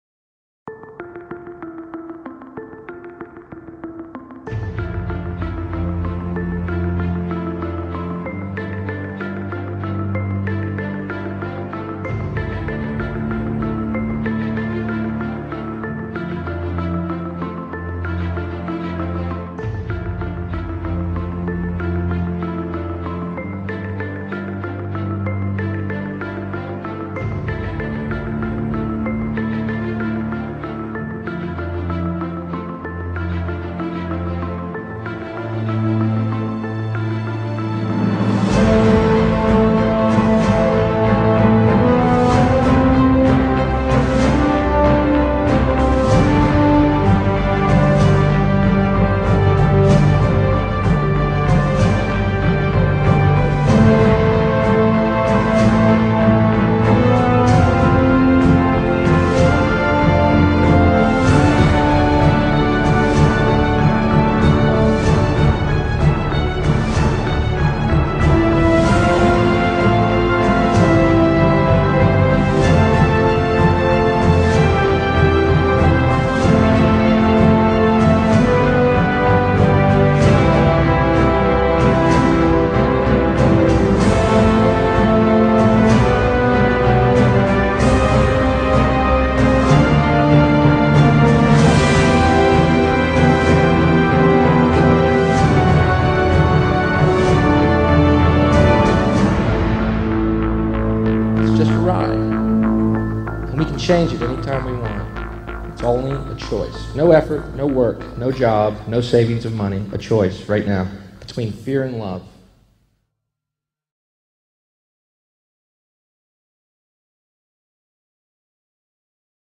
The Revolution Is Now
We Destroy the earth for the Sake of the Economy

We Destroy each other for the Sake of the Economy

We Indebt the poor for the Sake of the Economy

We are being kept asleep for the Sake of the Economy

but, A Shift in consciousness is happening on Planet Earth...

A Mass Awakening is Increasingly pushing the Boundaries of Society.